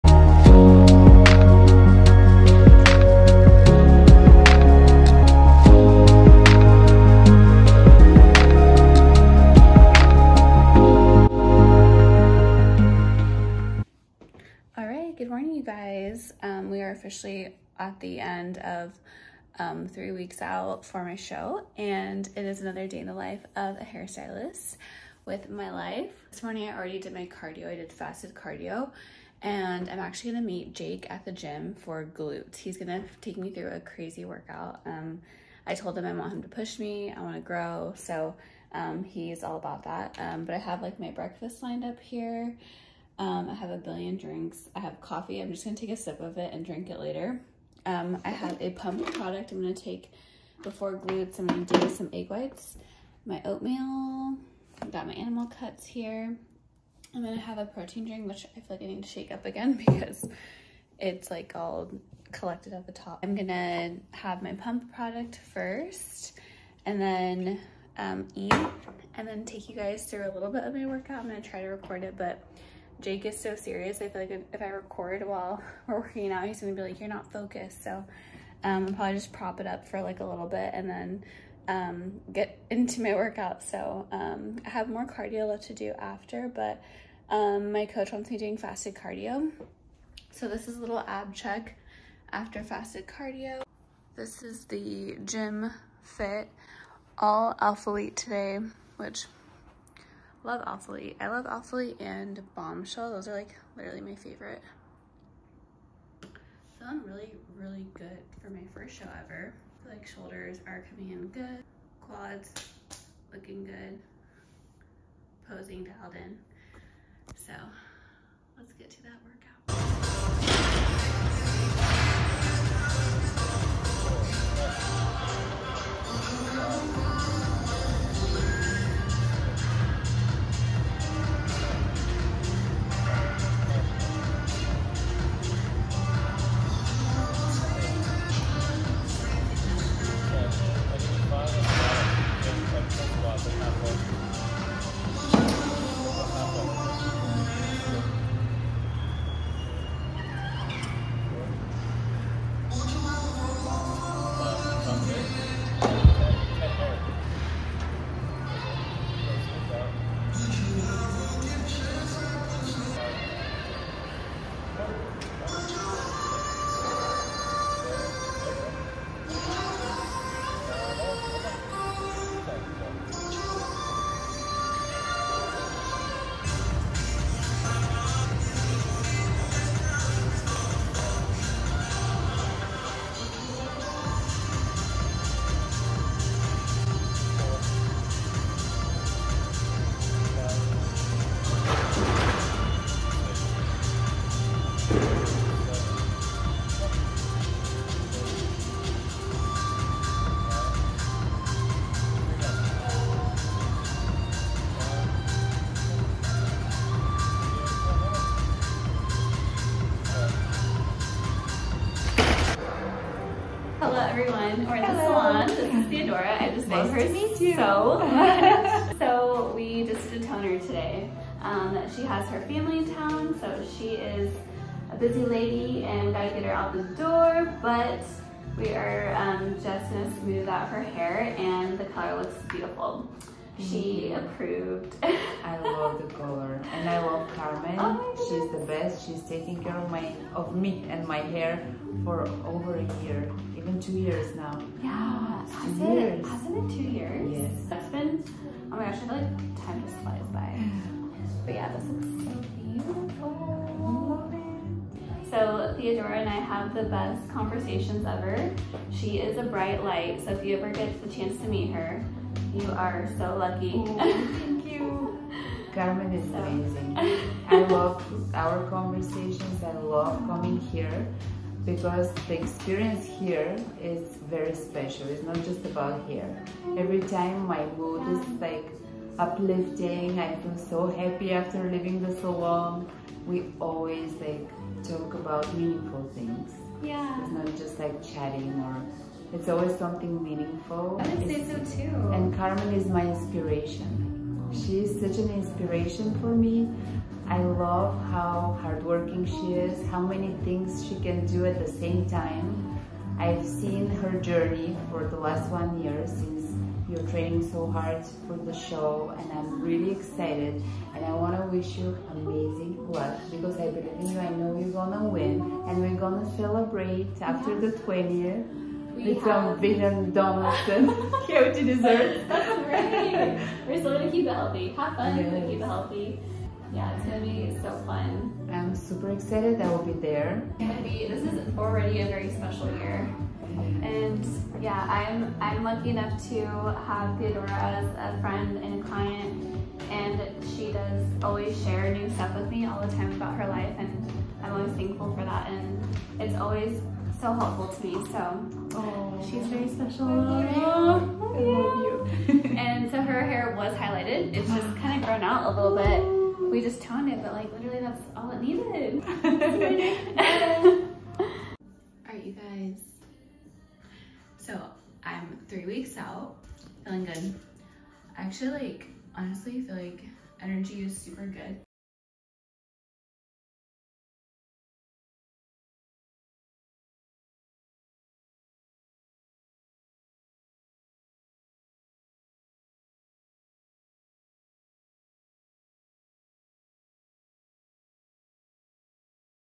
Jammin' in the Salon + GYM timeee!
Hi HAIR FAM! We're jammin' in the salon, thanking god for this life and entering just a little less than 3 WEEKS OUT from the 2024 NPC LA Muscle Contest Show!

Starting the day off with cardio, glutes and hamstring work with Jake and then servicing clients at NIXIE HAIR LOUNGE in Orange County.

& feeling so blessed to be able to offer color/extensions/blonding to amazing clients like you all. :)

I also received my Wellness Bikini from Toxic Anglez Bikinis and i'm OBSESSED!